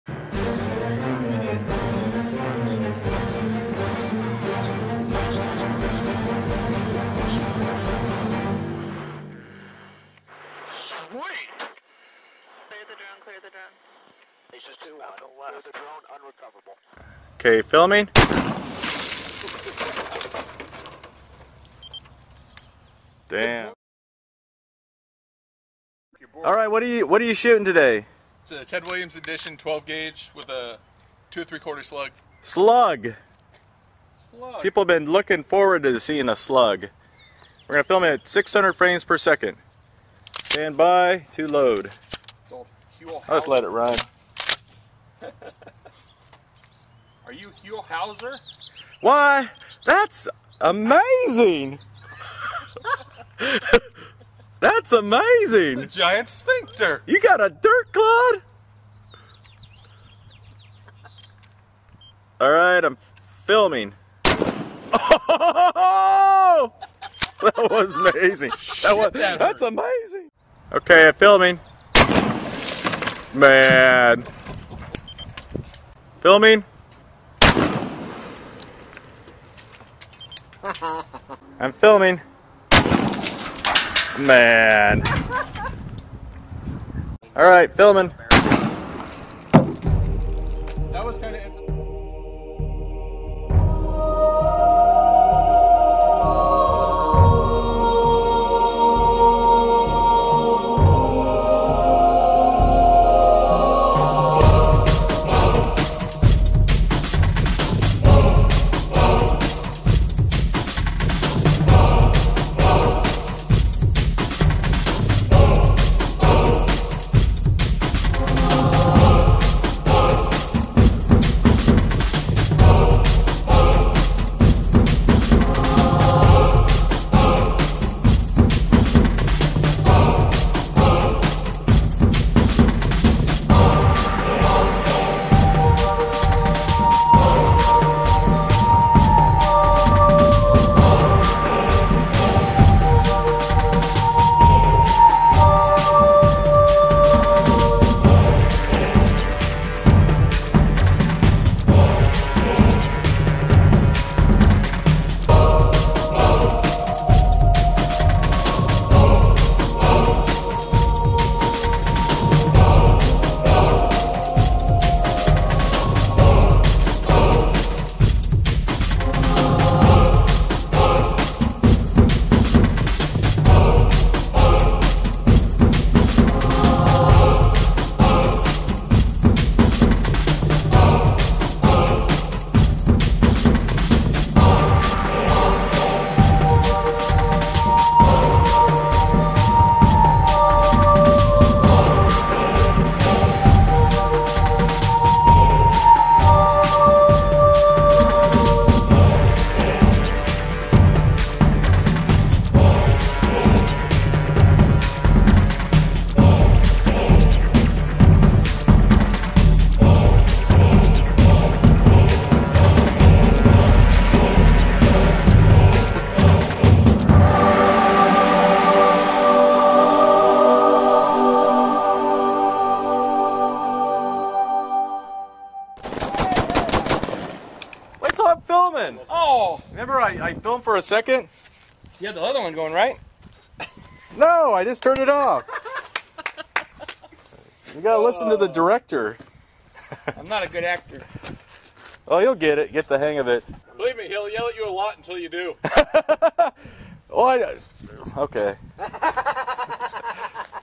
Slow Mo 12ga Shotgun Slug DESTRUCTION
We destroy water-filled jugs,  hard drive, cookie jar, toys, etc with powerful shotgun slugs.   Have fun!

High speed camera:  Casio EX-F1 Pro  600/1200 fps
Slug velocity:  1600+ Feet/Sec.
We shot a variety of objects and show the devastating power of a 1 ounce slug.